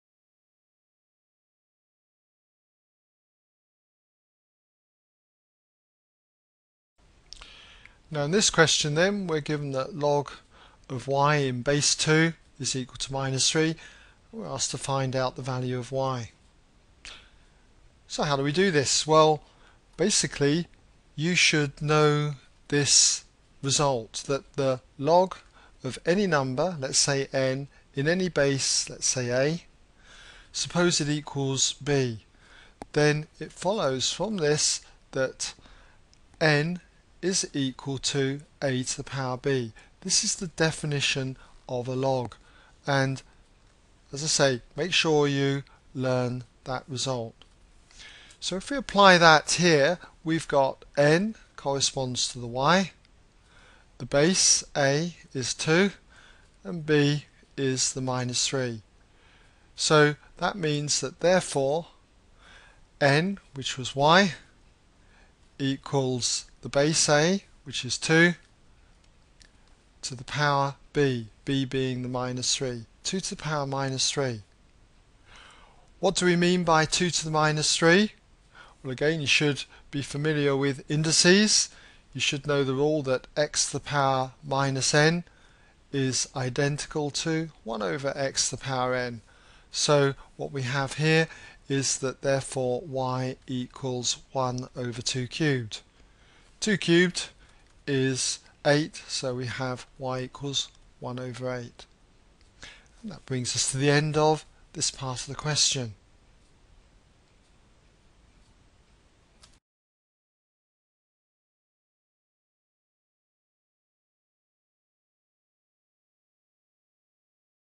A-Level Edexcel C2 June 2009 Q8(a) : ExamSolutions
Worked solution to this question on solving log equations. To see the question go to ExamSolutions